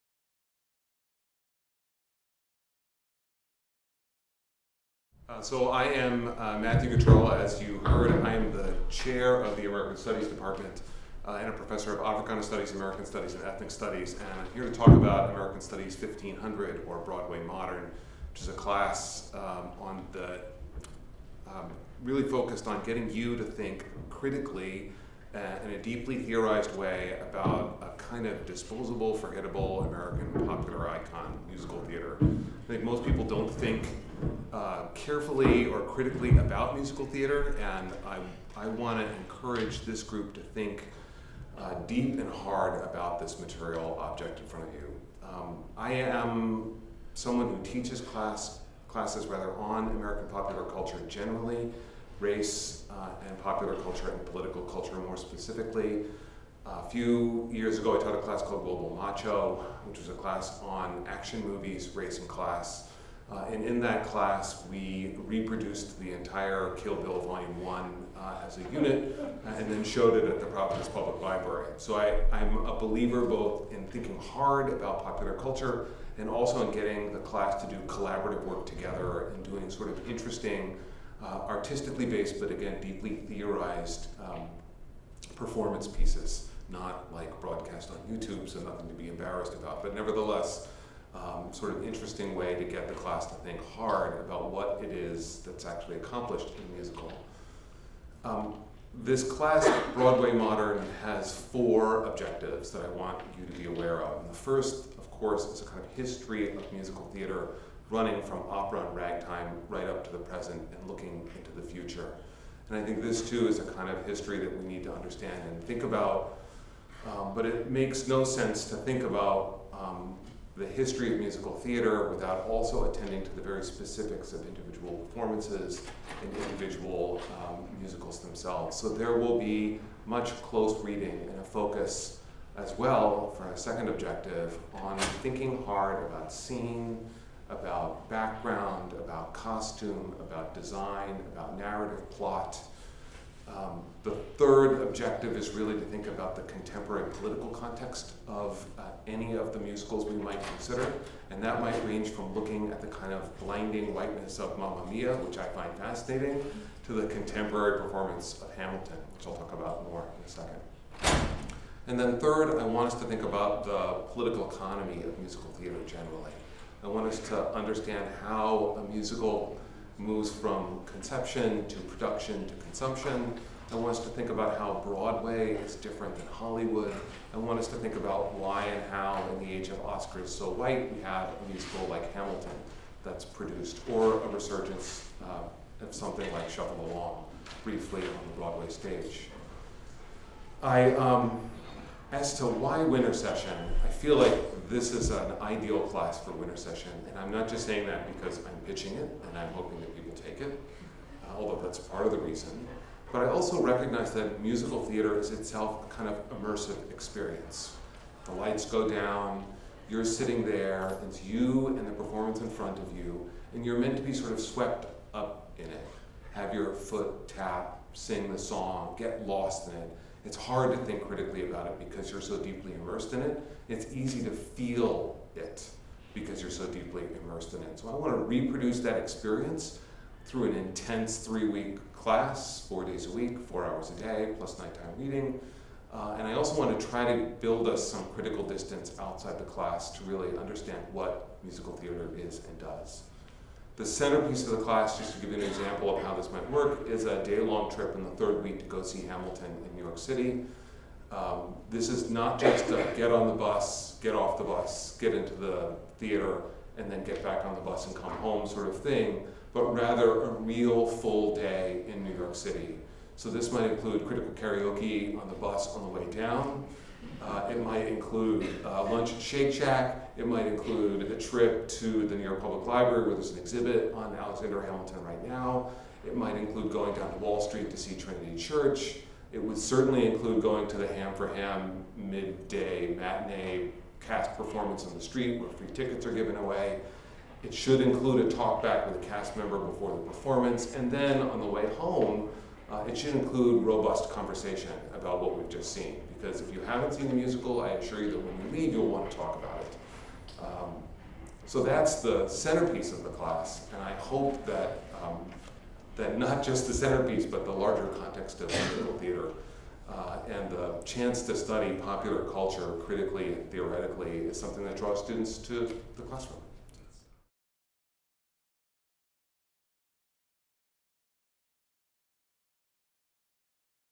Broadway Modern with Matthew Guterl - AMST1500B Wintersession
This course asks students to survey, critique, and theorize what the musical as a genre tells us about the interplay of race, sex, and class in contemporary American culture. The course includes a mix of critical readings on the history of the musical genre, on performance and identity, and on the Broadway musical Hamilton, which serves as an important centerpiece for the class.

AMST 1500B - Broadway Modern: Race, Sex, and Class in the Modern American Musical
Department: American Studies
Professor: Matthew Guterl
Dates: January 3 - January 20, 2017 (Travel Day to see Hamilton in New York City: January 17)